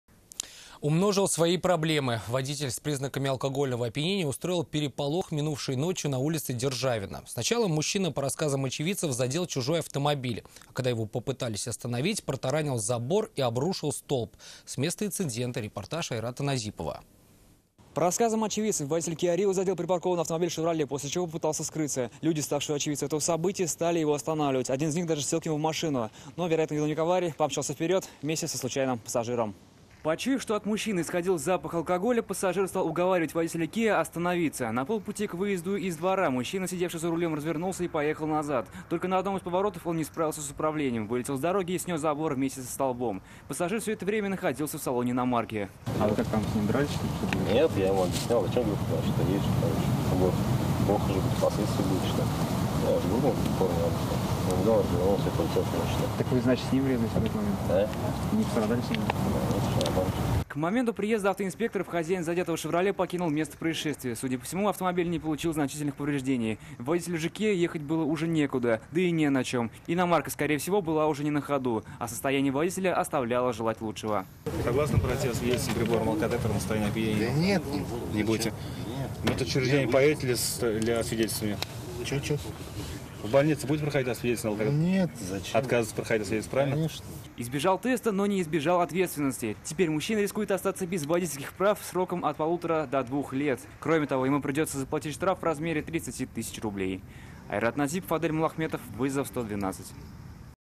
Протаранил забор и обрушил столб - ТНВ
Умножил свои проблемы. Водитель с признаками алкогольного опьянения устроил переполох минувшей ночью на улице Державина. Сначала мужчина, по рассказам очевидцев задел чужой автомобиль, а когда его попытались остановить - протаранил забор и обрушил столб.

Координаты:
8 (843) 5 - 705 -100